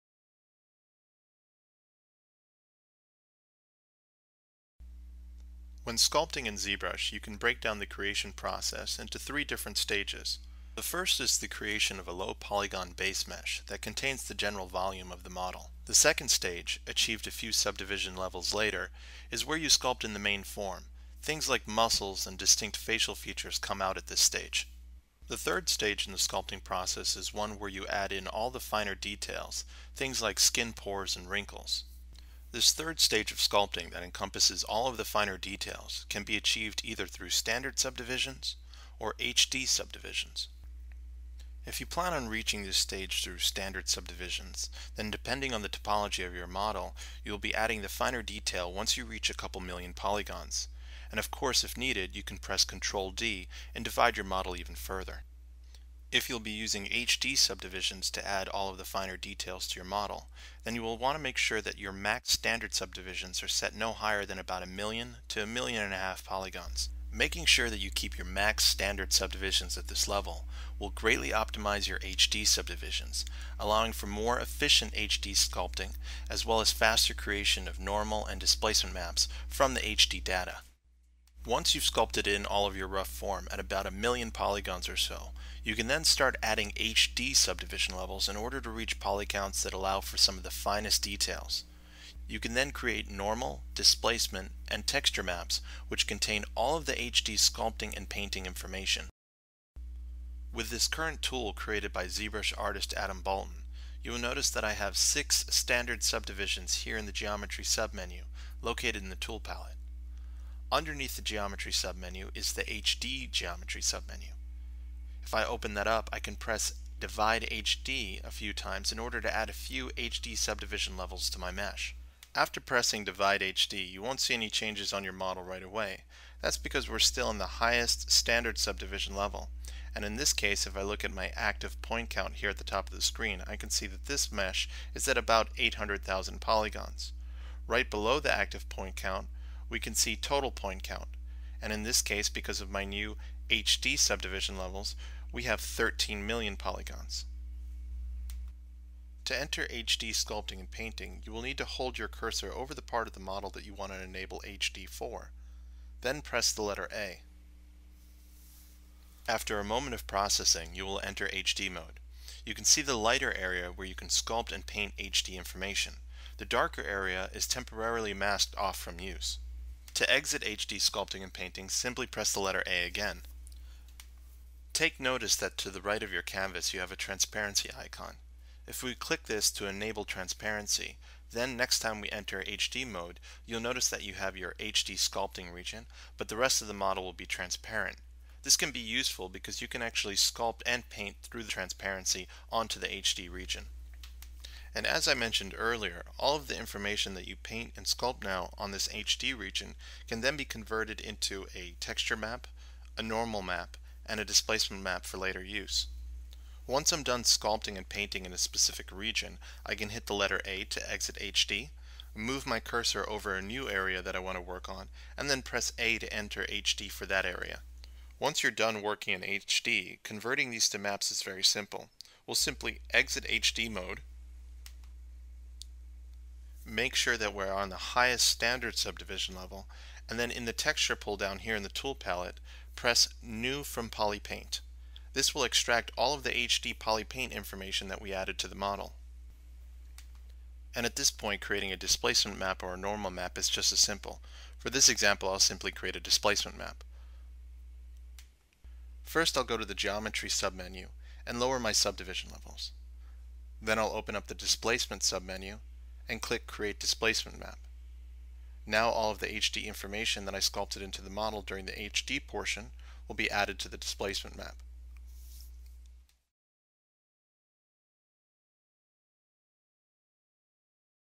Sculpting in HD
Learn to take advantage of the power of HD sculpting and painting. Sculpt and paint on up to a billion polygons, then convert your HD information to texture, normal, and displacement maps.